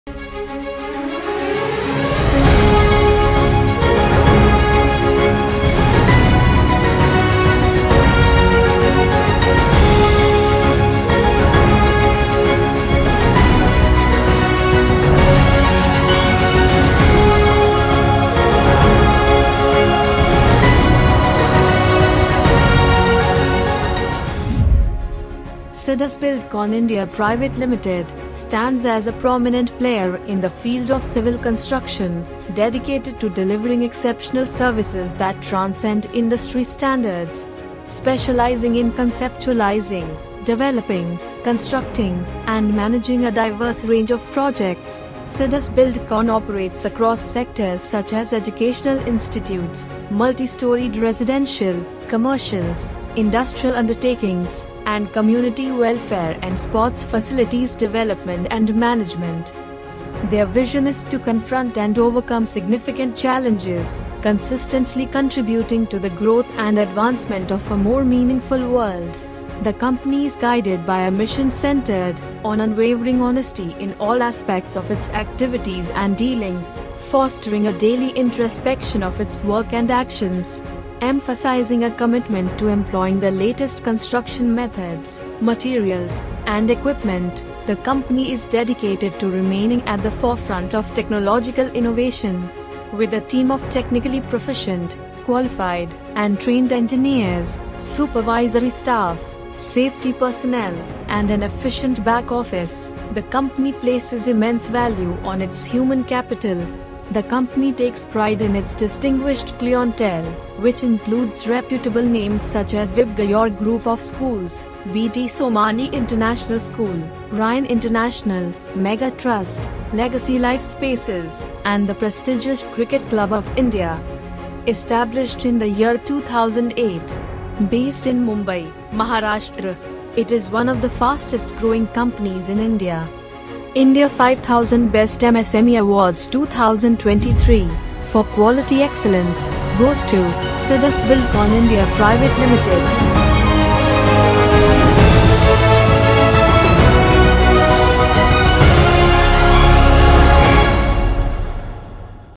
Sidus Buildcon India Pvt. Ltd. - Winner of India 5000 Best MSME Awards 2023.
Sidus Buildcon India Pvt. Ltd. stands as a prominent player in the field of civil construction, dedicated to delivering exceptional services that transcend industry standards. Specializing in conceptualizing, developing, constructing, and managing a diverse range of projects, Sidus Buildcon operates across sectors such as Educational Institutes, Multistoried Residential, Commercials, Industrial undertakings, and Community welfare and Sports Facilities development and management.

Their vision is to confront and overcome significant challenges, consistently contributing to the growth and advancement of a more meaningful world. The company is guided by a mission centered on unwavering honesty in all aspects of its activities and dealings, fostering a daily introspection of its work and actions. Emphasizing a commitment to employing the latest construction methods, materials, and equipment, the company is dedicated to remaining at the forefront of technological innovation. With a team of technically proficient, qualified, and trained engineers, supervisory staff, safety personnel, and an efficient back office, the company places immense value on its human capital. The company takes pride in its distinguished clientele, which includes reputable names such as Vibgyor Group of Schools, B.D. Somani International School, Ryan International, Mega Trust, Legacy Life Spaces, and the prestigious Cricket Club of India.

Established in the year of 2008, based in Mumbai, Maharashtra. It is one of the fastest growing companies in India.

India 5000 Best MSME Awards 2023, For Quality Excellence, Goes to, Sidus Buildcon India Pvt. Ltd.